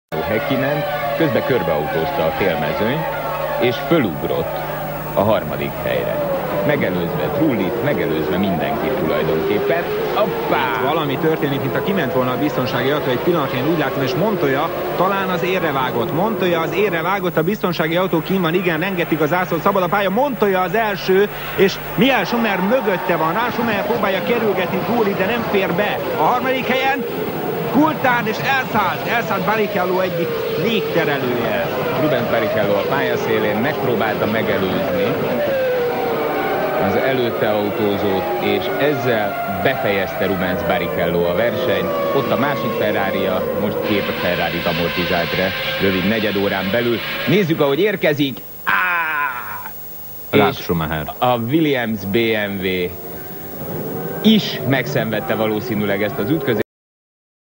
Palik (& Mezei Dániel) - Schumit leszorítja a pályáról Montoya
Montoya élet-halál harcot vív Schumacherrel, Rubens Barrichello pedig feltehetően világrekordot állított fel azzal, hogy cirka negyedóra leforgása alatt két Ferrarit tört rommá. Azért ez nem kis teljesítmény.